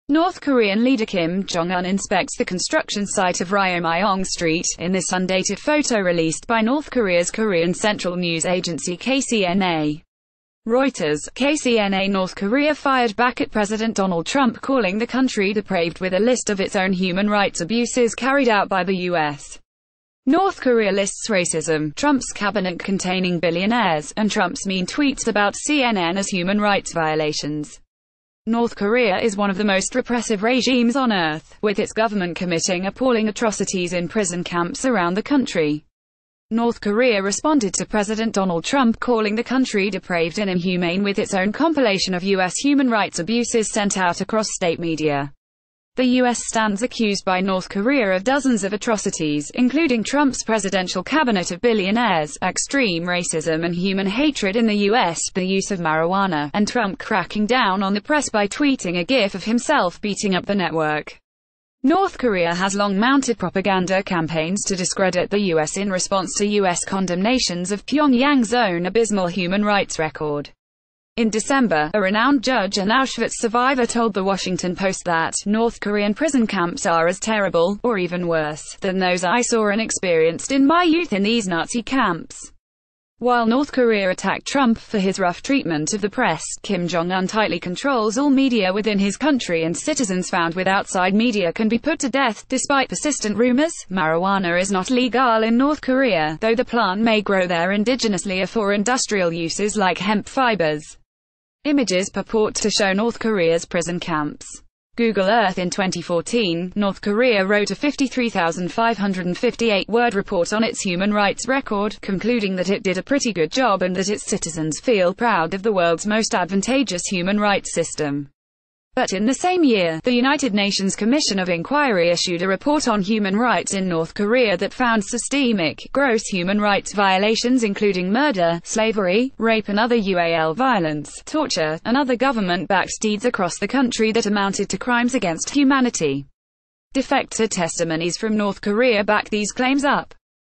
[Breaking News] North Korea bashes us for human rights abuses such as racial discrimination, mariju
[Breaking News] North Korea bashes us for human rights abuses such as racial discrimination, marijuana and Trump's tweets about CNN
North Korean leader Kim Jong Un inspects the construction site of Ryomyong Street, in this undated photo released by North Korea's Korean Central News Agency (KCNA). Reuters/KCNA North Korea fired back at President Donald Trump calling the country "depraved" with a list of its own human rights "abuses" carried out by the US. North Korea lists racism, Trump's cabinet containing billionaires, and Trump's mean tweets about CNN as human rights violations. North Korea is one of the most repressive regimes on earth, with its government committing appalling atrocities in prison camps around the country. North Korea responded to President Donald Trump calling the country "depraved" and inhumane with its own compilation of US human rights "abuses" sent out across state media. The US stands accused by North Korea of "dozens of atrocities" including Trump's presidential cabinet of billionaires, "extreme racism and human hatred" in the US, the use of marijuana, and Trump cracking down on the press by tweeting a gif of himself beating up the network. North Korea has long mounted propaganda campaigns to discredit the US in response to US condemnations of Pyongyang's own abysmal human rights record. In December, a renowned judge and Auschwitz survivor told the Washington Post that "[North] Korean prison camps are as terrible, or even worse, than those I saw and experienced in my youth in these Nazi camps." While North Korea attacked Trump for his rough treatment of the press, Kim Jong Un tightly controls all media within his country and citizens found with ...